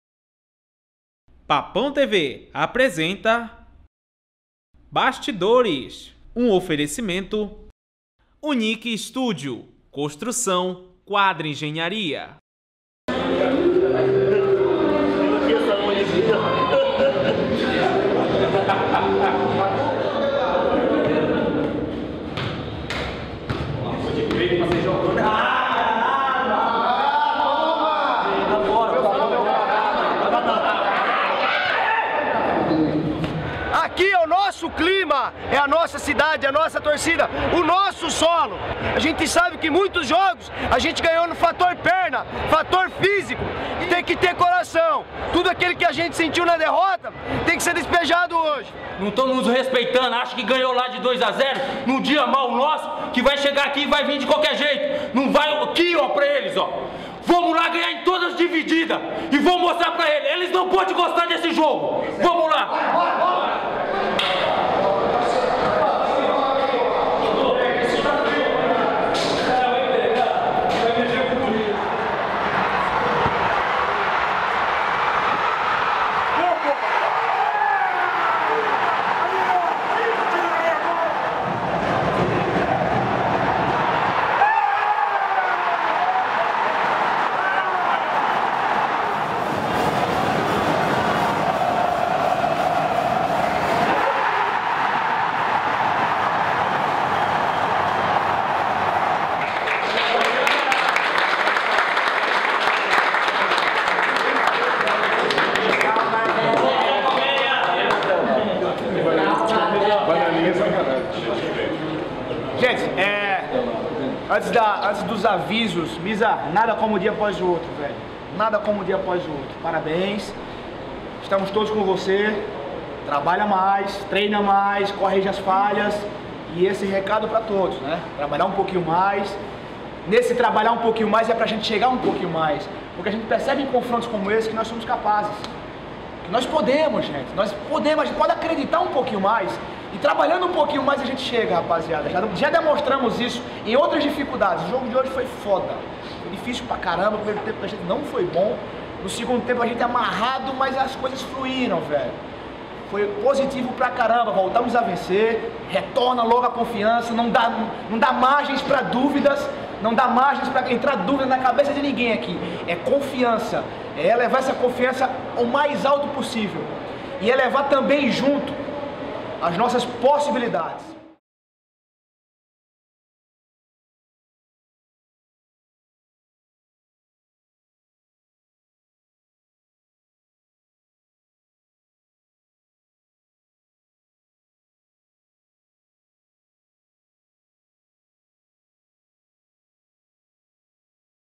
Bastidores #20 - Paysandu x Bahia (BA) - 16/07/2015
Veja tudo que rolou antes da vitória contra o Bahia (BA) pela Copa do Brasil, e saiba o que o técnico Dado Cavalcanti disse para o jogador Misael após a partida, em mais vídeo exclusivo da PapãoTV.